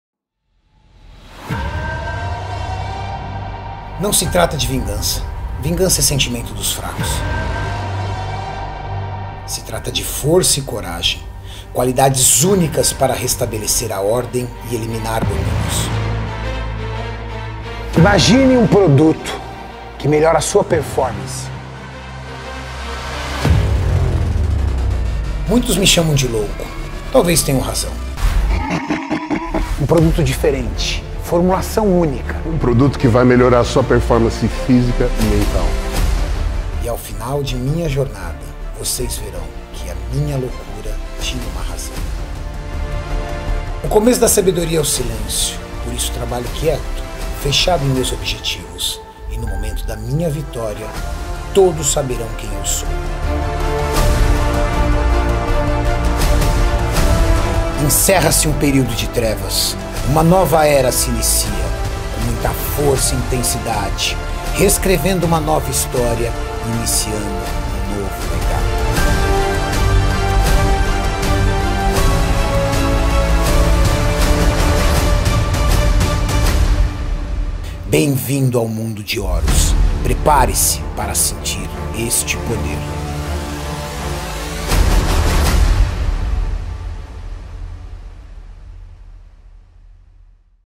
Pré Treino Horus Max Titanium
👇🏻 Nos sabores:

🍓 Frutas Vermelhas
💚 Limão Yuzu
💙 Blue Ice
🍊 Citrus
🍭 Cotton Candy

🥵 Para quem se exercita com intensidade, a fadiga é um dos principais fatores que influenciam o rendimento, por isso desenvolvemos o HÓRUS: com formulação altamente tecnológica e ingredientes de altíssima qualidade, para você ter um treino com muito
mais eficiência.

🔺 Benefícios:
🔝 Resistência
🔝 Desempenho

🔺Contém:
🔝 Beta Alanina 2g
🔝 Taurina 1.000mg
🔝 Arginina 1.000mg
🔝 Cafeína 150mg